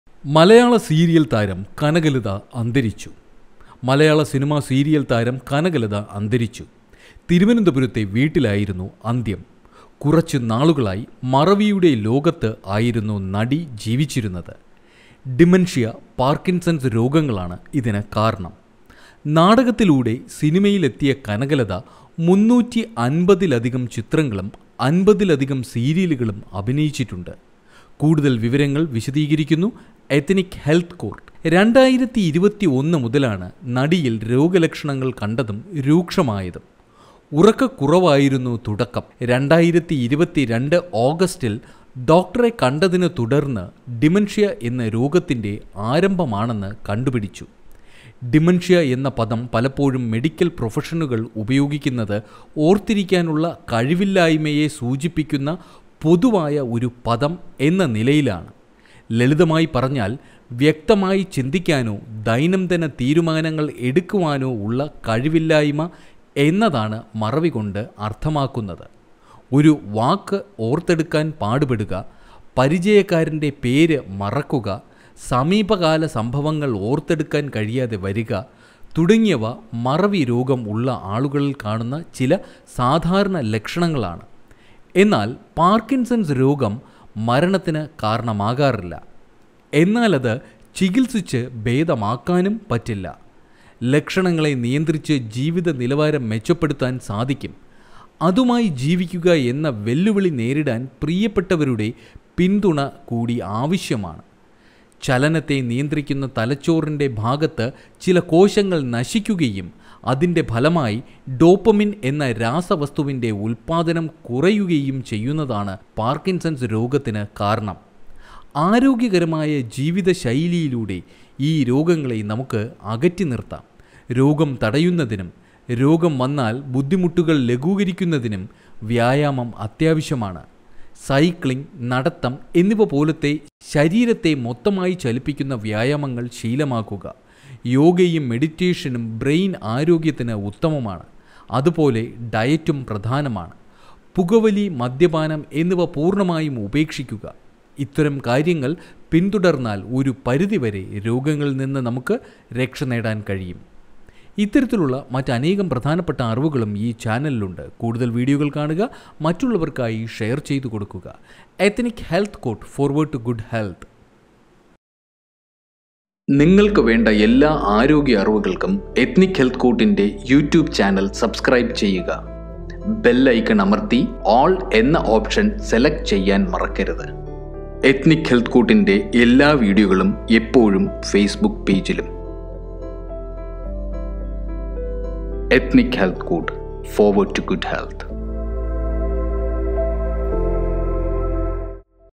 മലയാള സിനിമാ സീരിയല്‍ താരം കനകലത അന്തരിച്ചു.| Ethnic Health Court
മറവിരോഗവും പാര്‍ക്കിന്‍സണ്‍സും ബാധിച്ച് ചികിത്സയിലായിരുന്നു. 350-ലധികം ചിത്രങ്ങളിലും അമ്പതിലധികം സീരിയലുകളിലും കനകലത അഭിനയിച്ചിട്ടുണ്ട്.